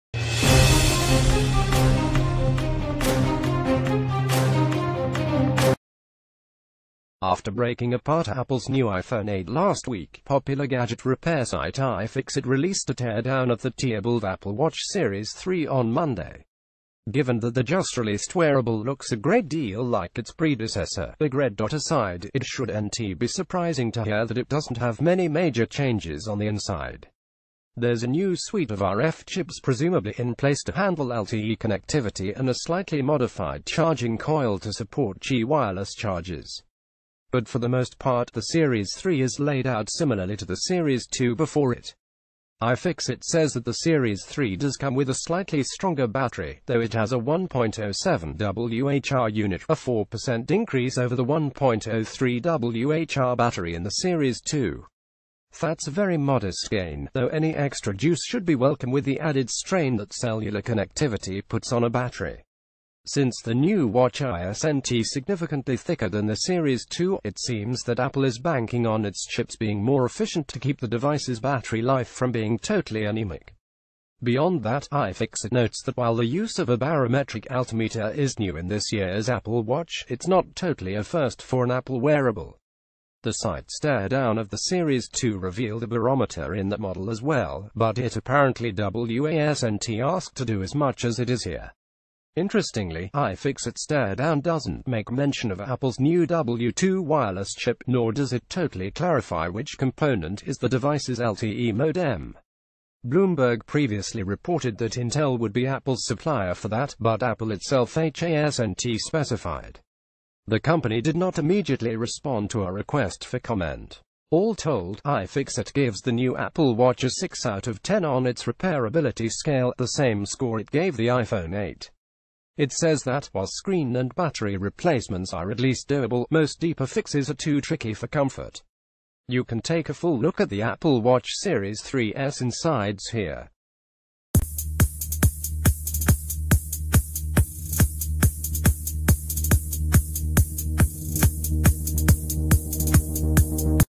Apple watch series 3 teardown finds a bigger battery and few surprises by BuzzFresh News
Apple watch series 3 teardown finds a bigger battery and few surprises